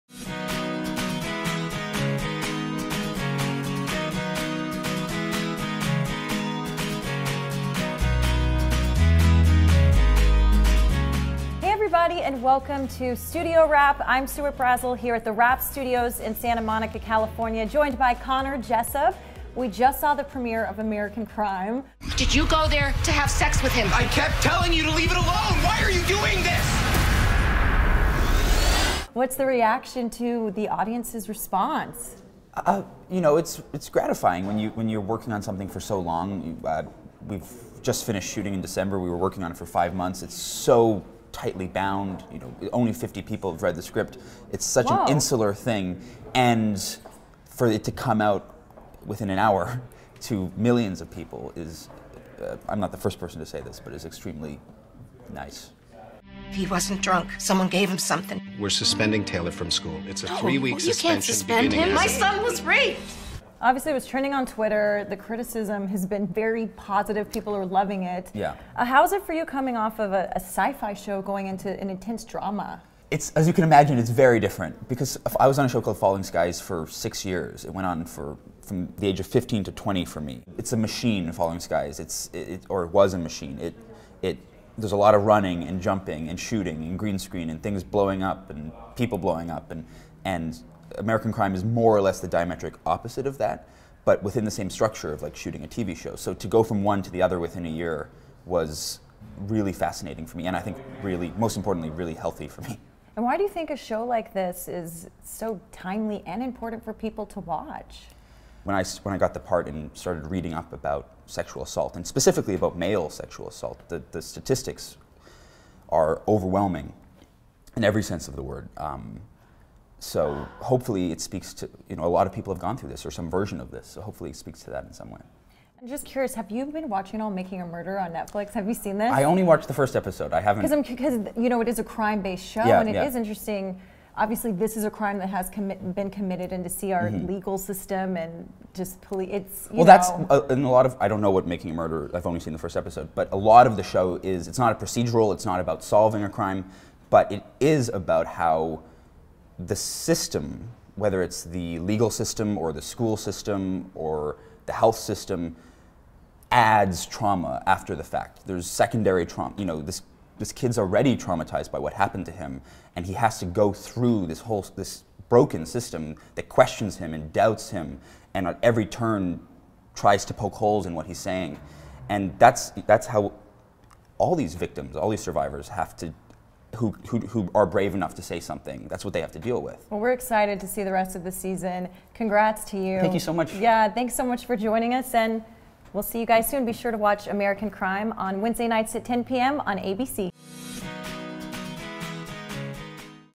'American Crime' Star Connor Jessup on Similarities to 'Making a Murderer'
"It is about how the system ... adds trauma after the fact," ABC actor tells TheWrap.